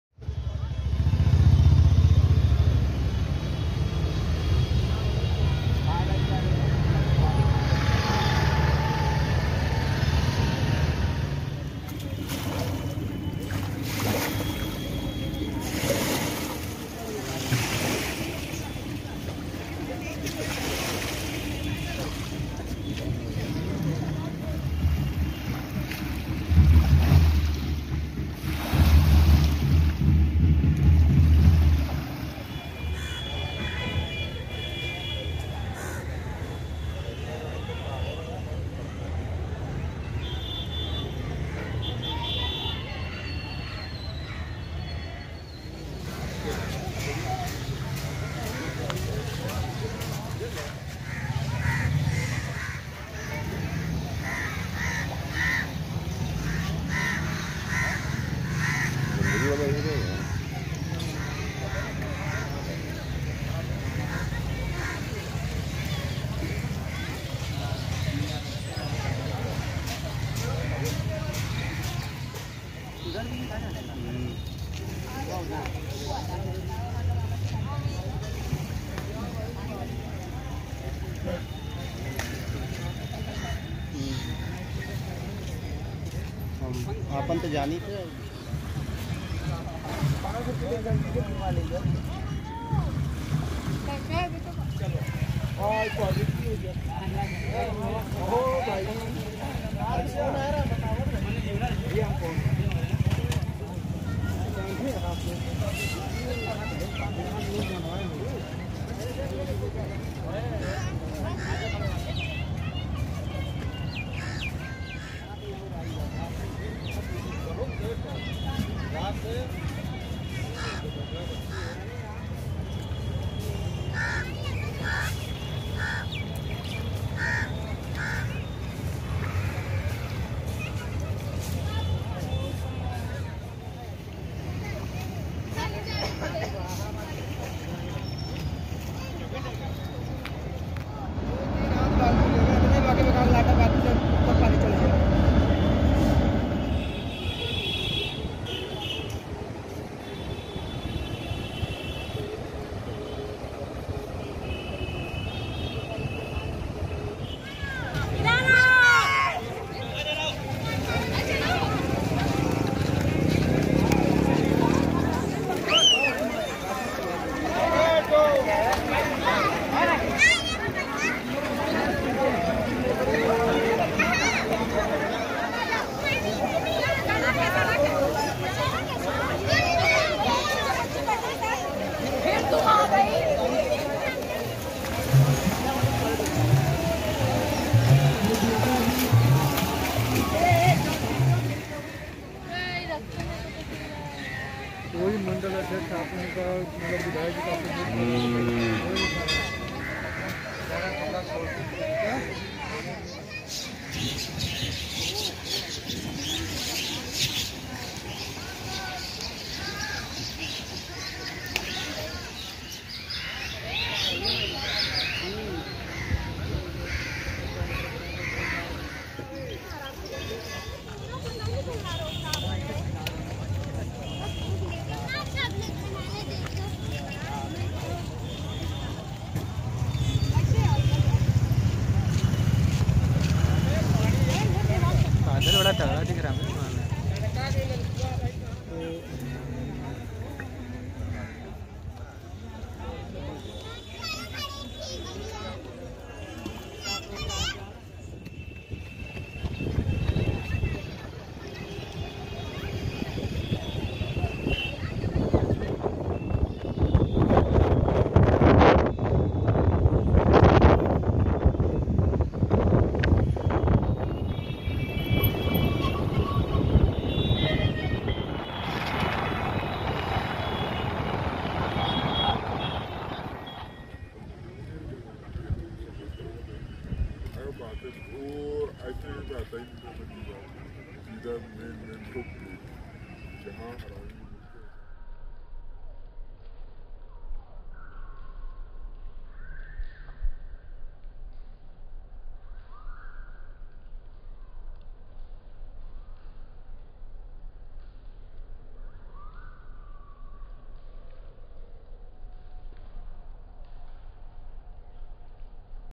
डुबता हुआ प्रयागराज || प्रयागराज में गंगा- यमुना का जल स्तर बढ़ाने से बढ़ का खतरा
Video shoot on: Samsung F62

श्रृंगवेरपुर धाम

चित्रकूट धाम दर्शन

prayagraj_HUB_91